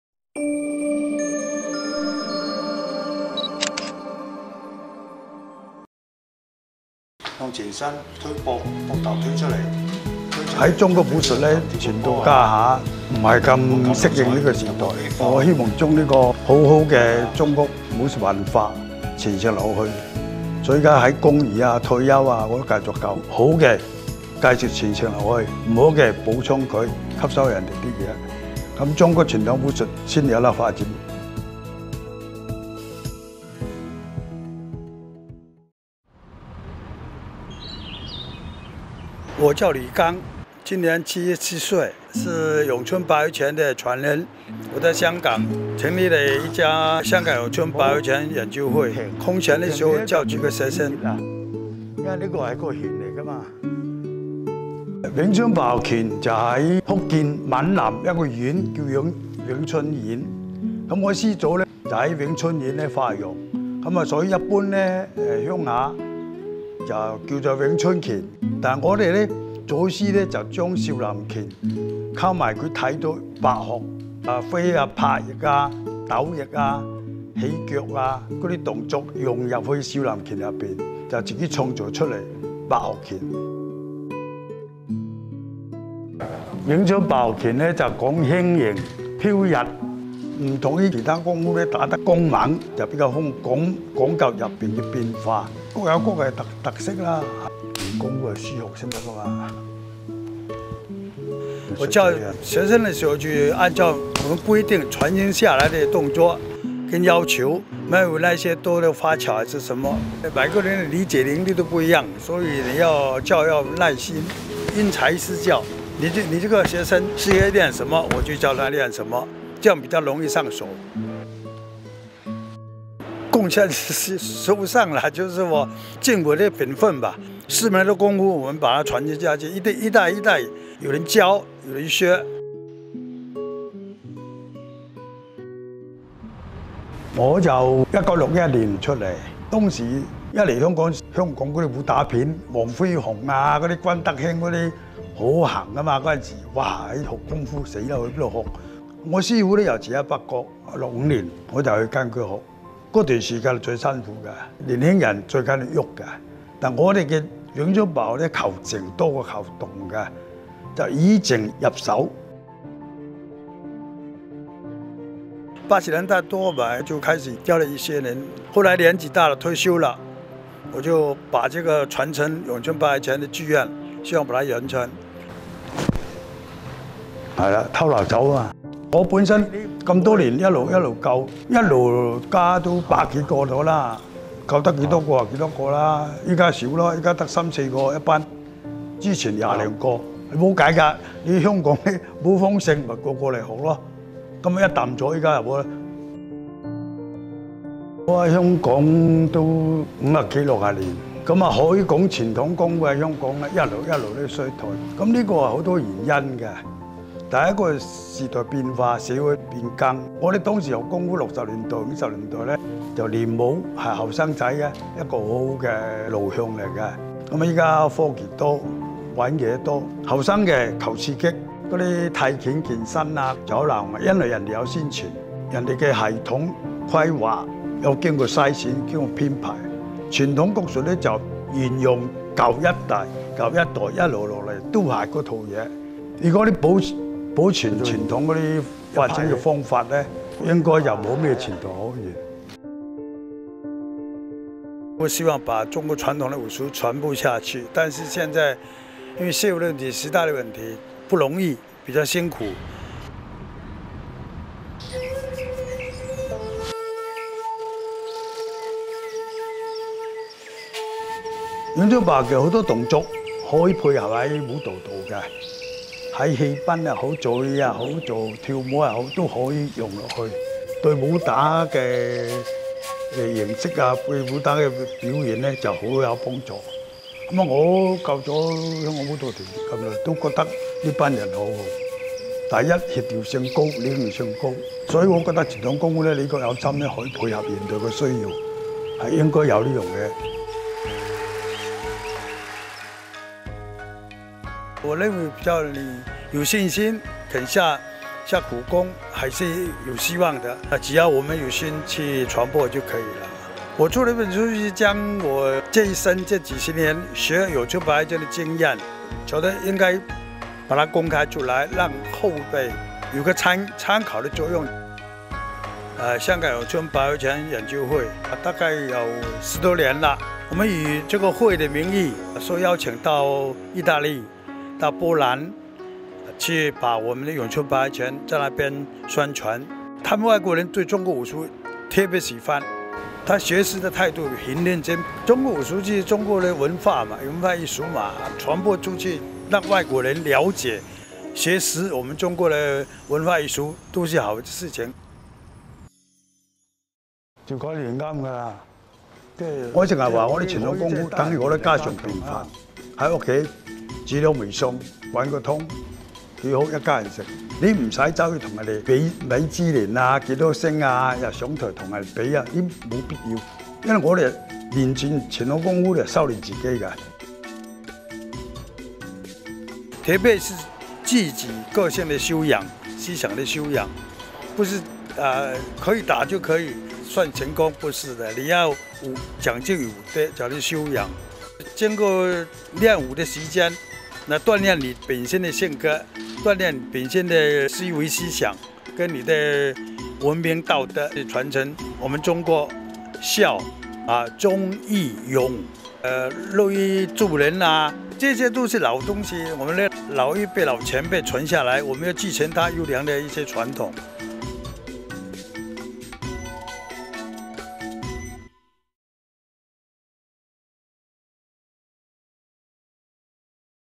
香港老拳师说学武不为能打 六十年见证传统武术兴衰 为传承大胆出新
77岁的李刚是永春白鹤拳传人，他年少离开福建老家，在香港拜师学拳初入“江湖”。此后近六十年的时光，他见证了传统武术在香港的兴衰，也致力于师门传承，他教拳、出书、赴海外宣传，他不断创新表演形式，只为寻一条传承之路。
他坚信，学武并非为了上台比拼，而是修炼自身，继承先辈们留下的优良传统。